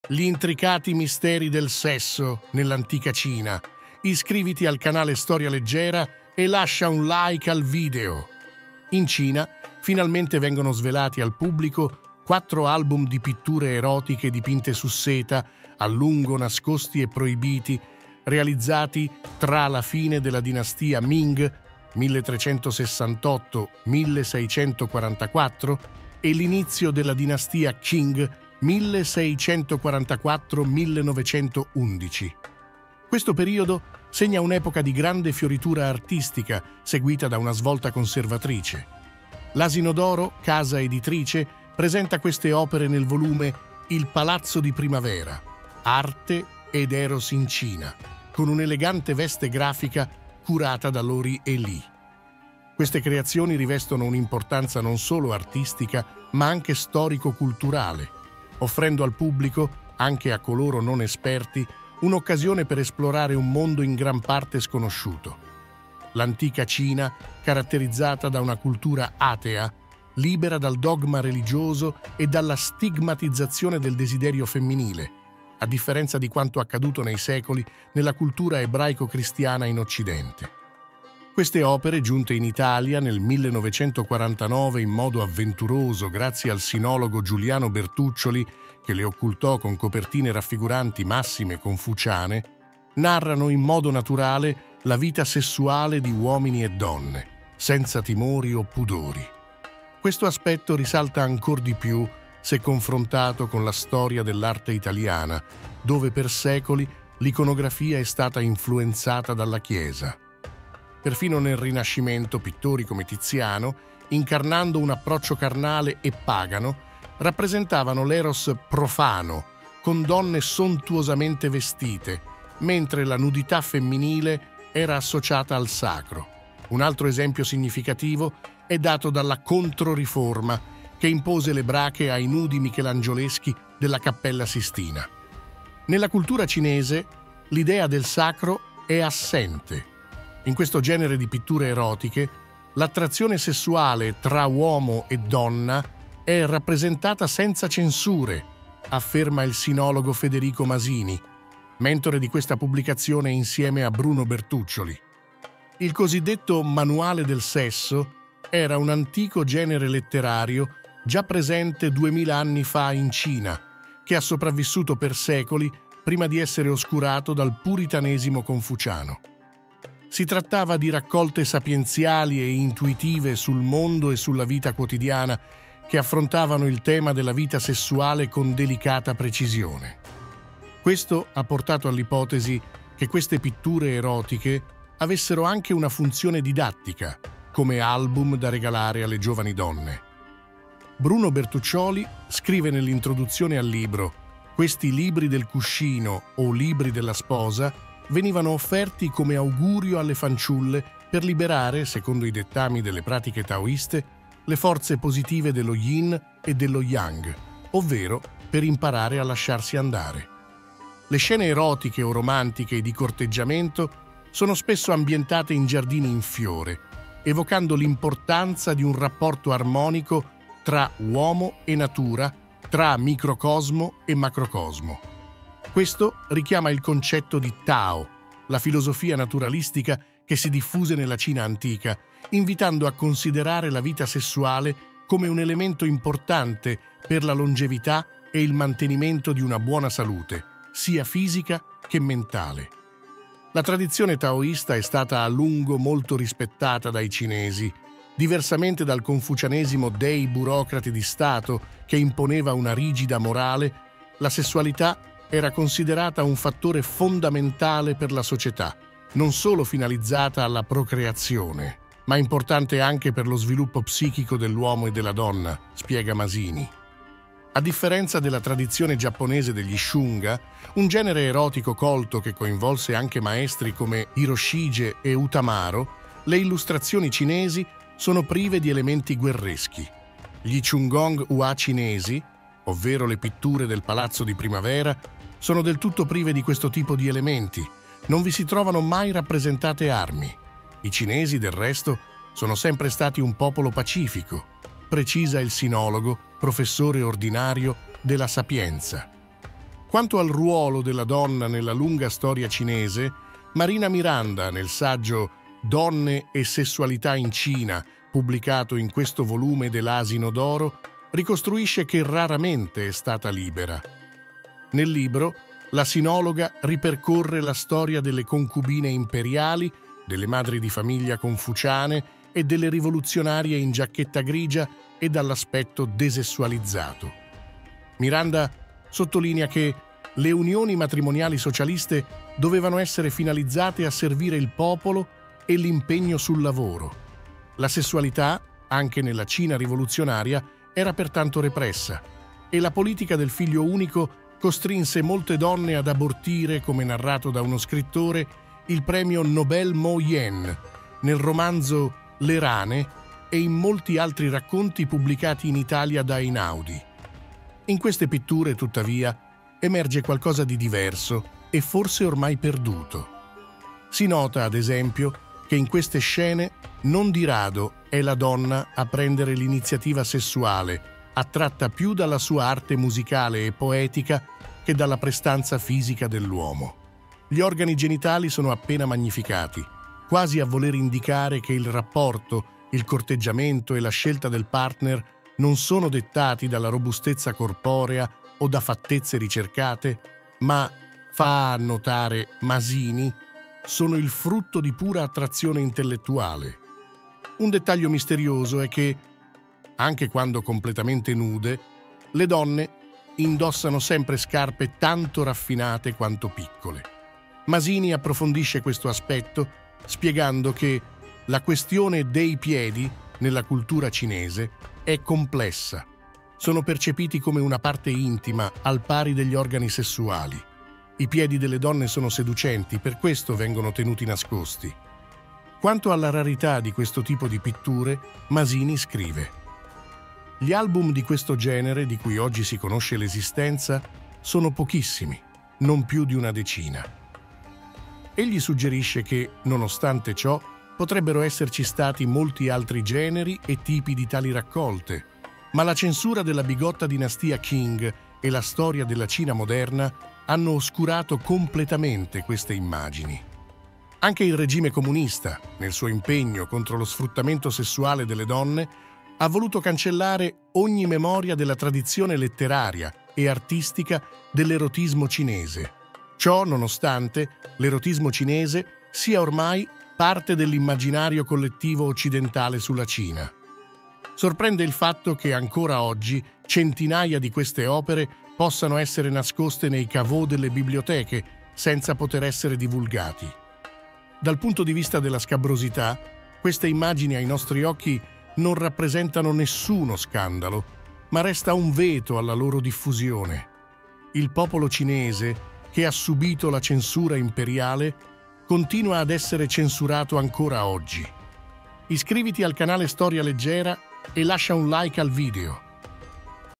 Sesso nell'antica Cina
In questo video parliamo del sesso nell'antica Cina.